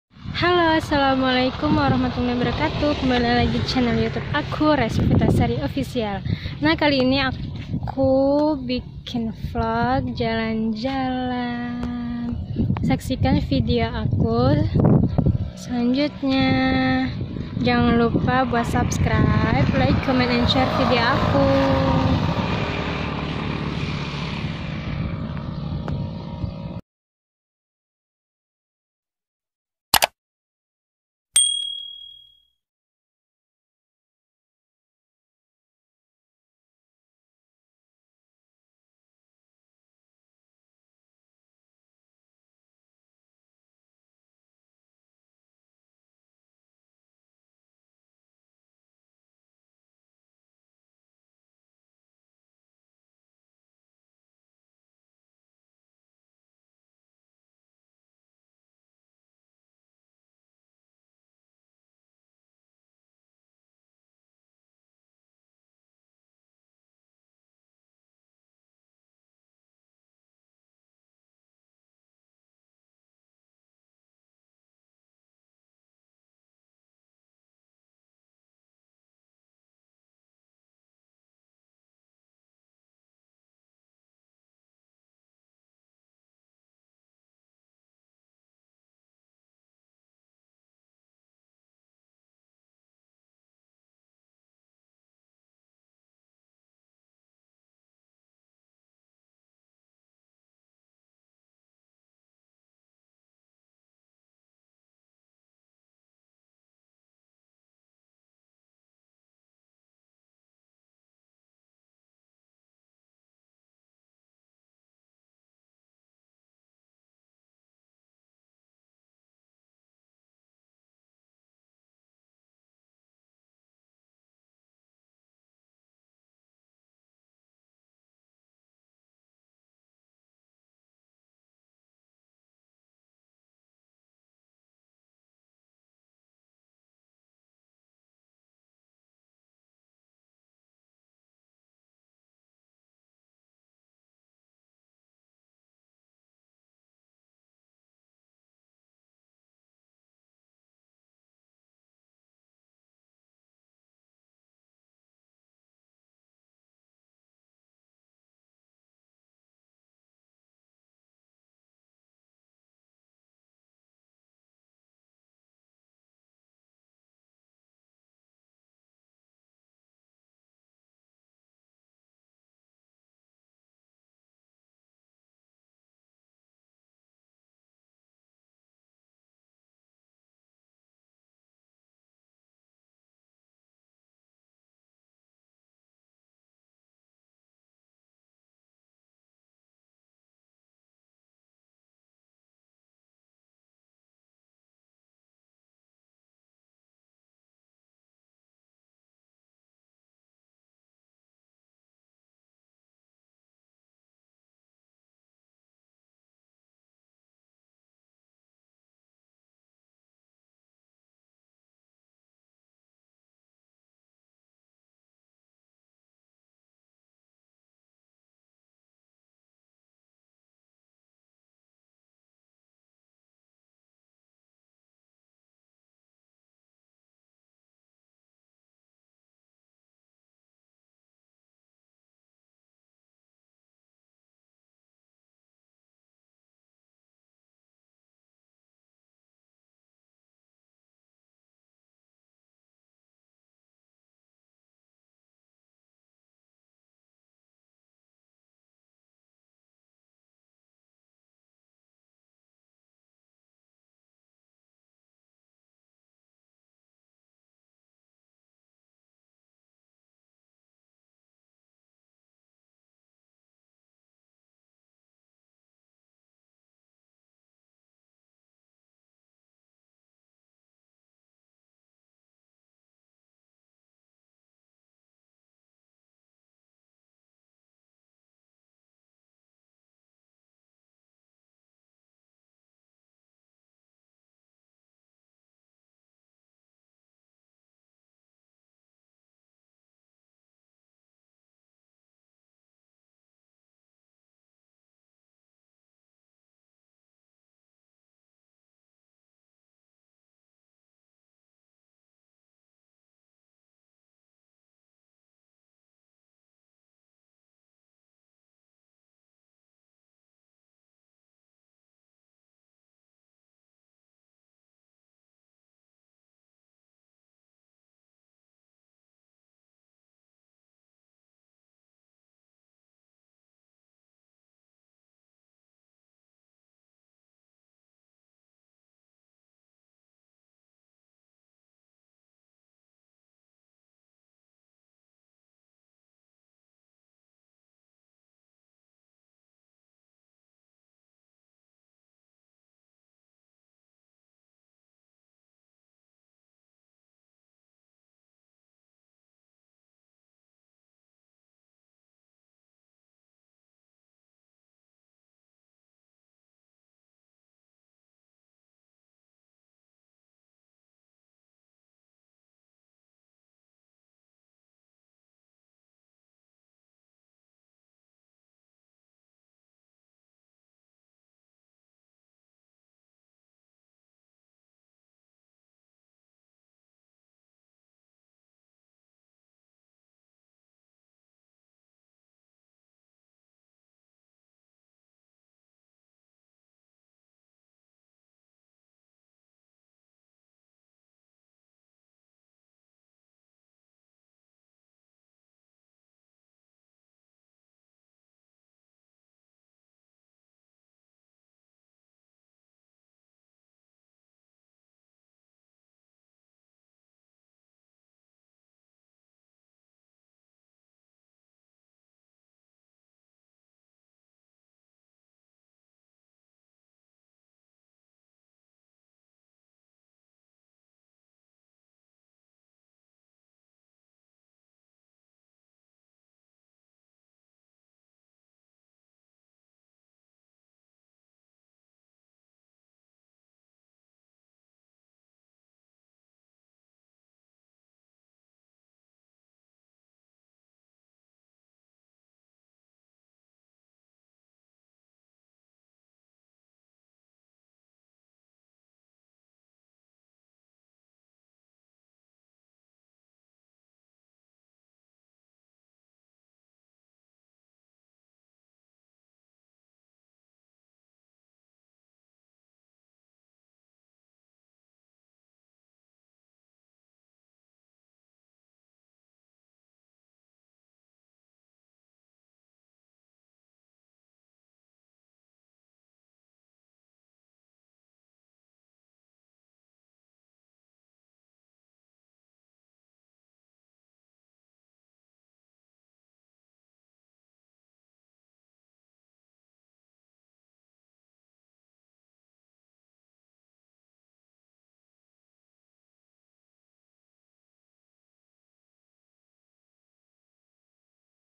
LIBURAN SETELAH MENIKAH || Part 1
Hari yang ditunggu saat liburan bersama suami, setelah 5 bulan baru bisa jalan² sama dia,karena banyaknya urusan yang membuat semuanya menghambat 😂it's oke nggak papa ,rasanya senang banget bisa liburan.